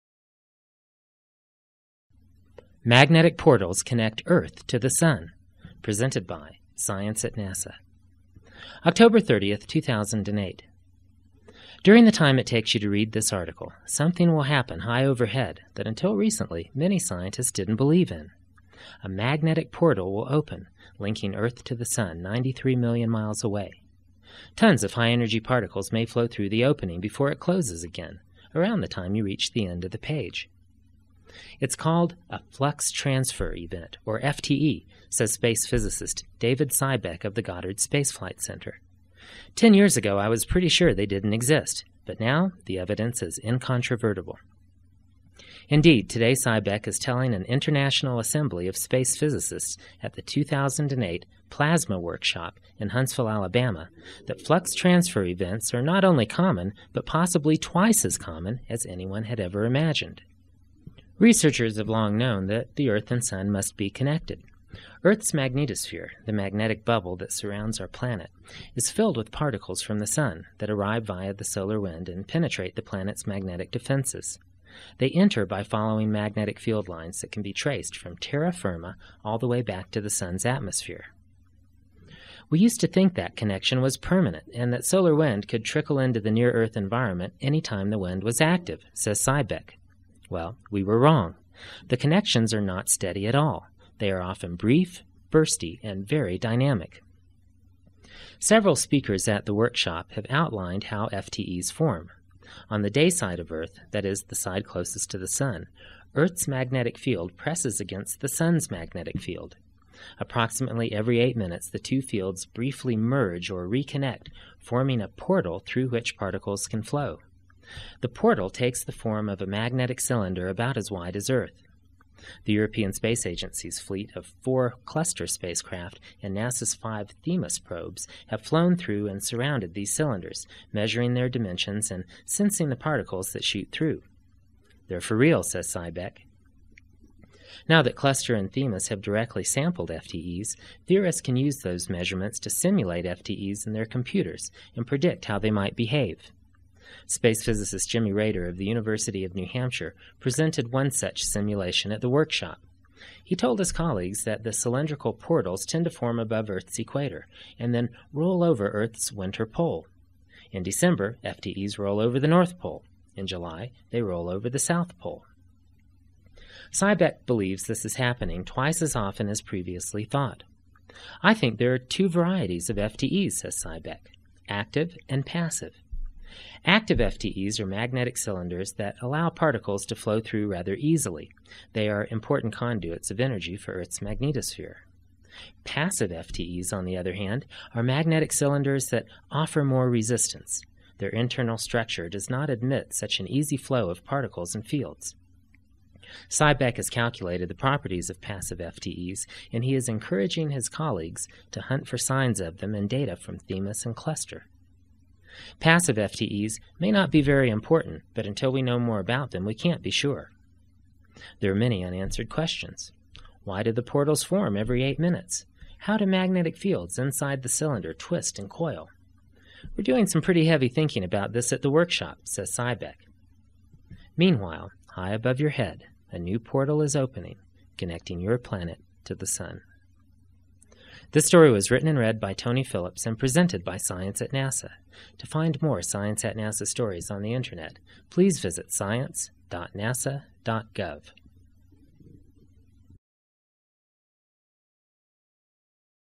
2008-10-30 - SCIENCE.NASA.GOV - MAGNETIC PORTALS CONNECT EARTH TO SUN
Oct. 30, 2008: During the time it takes you to read this article, something will happen high overhead that until recently many scientists didn't believe in. A magnetic portal will open, linking Earth to the sun 93 million miles away. Tons of high-energy particles may flow through the opening before it closes again, around the time you reach the end of the page.

"It's called a flux transfer event or 'FTE,'" says space physicist David Sibeck of the Goddard Space Flight Center. "Ten years ago I was pretty sure they didn't exist, but now the evidence is incontrovertible."

Indeed, today Sibeck is telling an international assembly of space physicists at the 2008 Plasma Workshop in Huntsville, Alabama, that FTEs are not just common, but possibly twice as common as anyone had ever imagined.

Researchers have long known that the Earth and sun must be connected. Earth's magnetosphere (the magnetic bubble that surrounds our planet) is filled with particles from the sun that arrive via the solar wind and penetrate the planet's magnetic defenses. They enter by following magnetic field lines that can be traced from terra firma all the way back to the sun's atmosphere.

"We used to think the connection was permanent and that solar wind could trickle into the near-Earth environment anytime the wind was active," says Sibeck. "We were wrong. The connections are not steady at all. They are often brief, bursty and very dynamic."

Several speakers at the Workshop have outlined how FTEs form: On the dayside of Earth (the side closest to the sun), Earth's magnetic field presses against the sun's magnetic field. Approximately every eight minutes, the two fields briefly merge or "reconnect," forming a portal through which particles can flow. The portal takes the form of a magnetic cylinder about as wide as Earth. The European Space Agency's fleet of four Cluster spacecraft and NASA's five THEMIS probes have flown through and surrounded these cylinders, measuring their dimensions and sensing the particles that shoot through. "They're real," says Sibeck.

Now that Cluster and THEMIS have directly sampled FTEs, theorists can use those measurements to simulate FTEs in their computers and predict how they might behave. Space physicist Jimmy Raeder of the University of New Hampshire presented one such simulation at the Workshop. He told his colleagues that the cylindrical portals tend to form above Earth's equator and then roll over Earth's winter pole. In December, FTEs roll over the north pole; in July they roll over the south pole.

Sibeck believes this is happening twice as often as previously thought. "I think there are two varieties of FTEs: active and passive." Active FTEs are magnetic cylinders that allow particles to flow through rather easily; they are important conduits of energy for Earth's magnetosphere. Passive FTEs are magnetic cylinders that offer more resistance; their internal structure does not admit such an easy flow of particles and fields. (For experts: Active FTEs form at equatorial latitudes when the IMF tips south; passive FTEs form at higher latitudes when the IMF tips north.) Sibeck has calculated the properties of passive FTEs and he is encouraging his colleagues to hunt for signs of them in data from THEMIS and Cluster. "Passive FTEs may not be very important, but until we know more about them we can't be sure."

There are many unanswered questions: Why do the portals form every 8 minutes? How do magnetic fields inside the cylinder twist and coil? "We're doing some heavy thinking about this at the Workshop," says Sibeck.

Meanwhile, high above your head, a new portal is opening, connecting your planet to the sun.